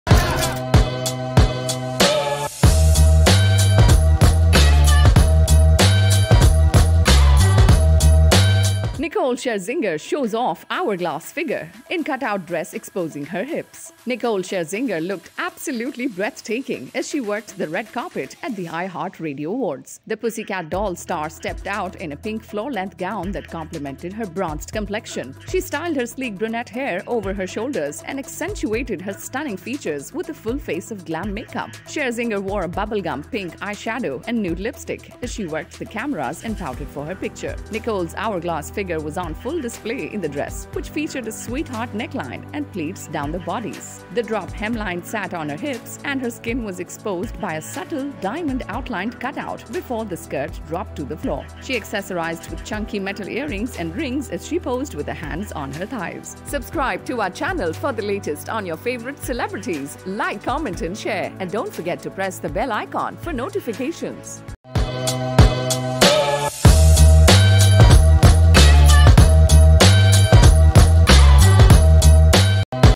Nicole Scherzinger shows off hourglass figure in cut out dress exposing her hips
Nicole Scherzinger has put her incredible figure on display in a gorgeous floor length pink cut out dress that exposed her hip and made her look a million dollars

nicole scherzinger | scherzinger | nicole | nicole scherzinger vevo | nicole scherzinger live